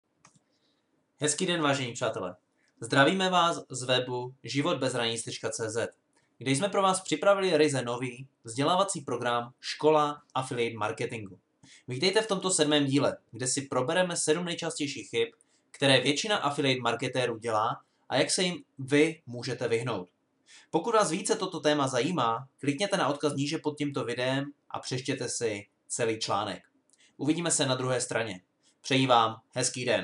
Affiliate marketing: 7 Nejčastějších chyb affiliate marketérů
Podívejte se na těchto 7 nejčastějších chyb, které většina affiliate marketérů dělá, a jak se jim vyhnout.